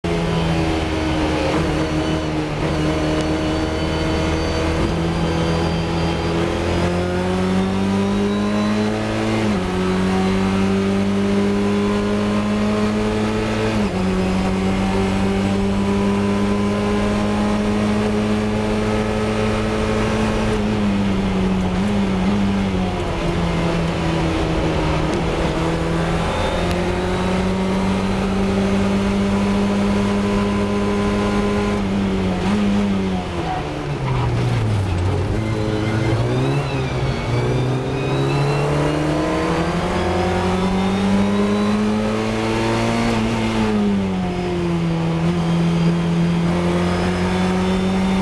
Safety Car re-start brands Hatch FunCup April 2016
Great dive at paddock, shame I lost out at Druids.